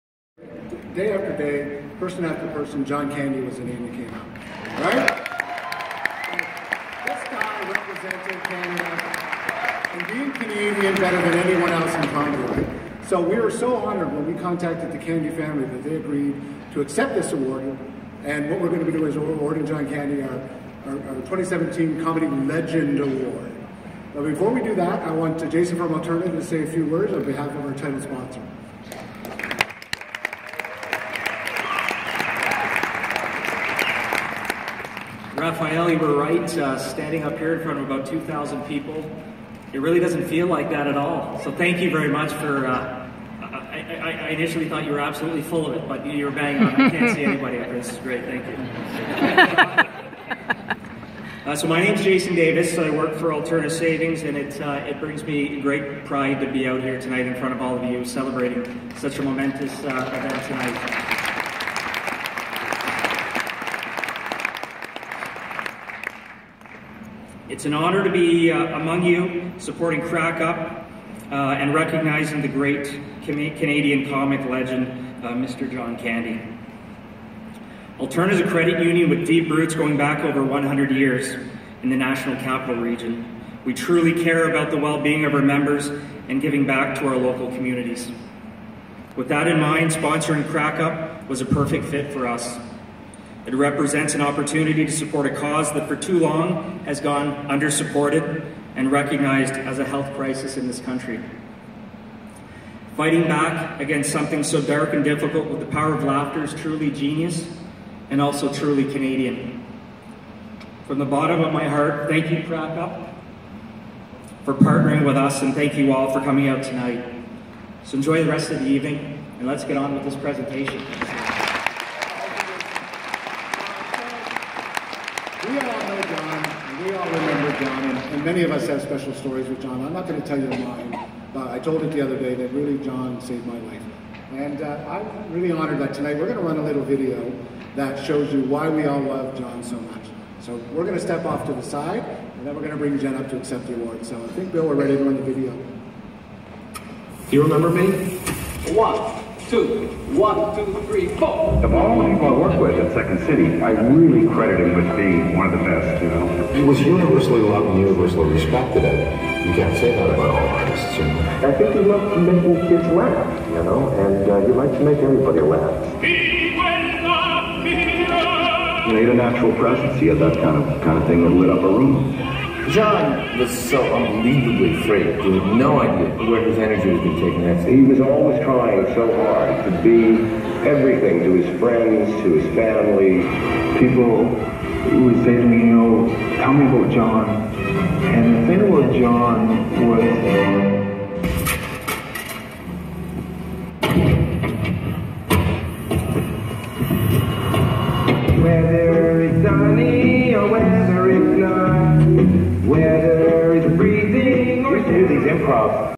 Honouring John Candy at Alterna Savings Crackup Comedy Night In Canada
Ottawa 2017 - Awarding John Candy (via Jen Candy) the Canadian Comedy Icon Award